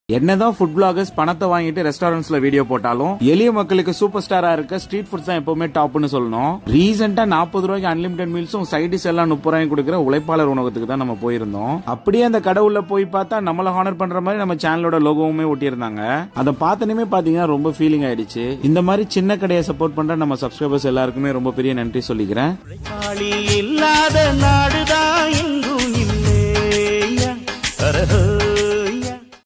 💥🤤 உழைப்பாளர்களின் 40₹ Unlimited Meals - Tamil Food Review
Ulaipalargalin Unavagam
Pidariyar, Koil St,
George Town, Parrys,